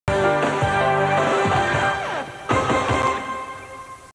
KKTV Station ID January 1993
This is KKTV's station ID from CBS's 1992-93 season "This is CBS"
KKTV is located in Colorado Springs, CO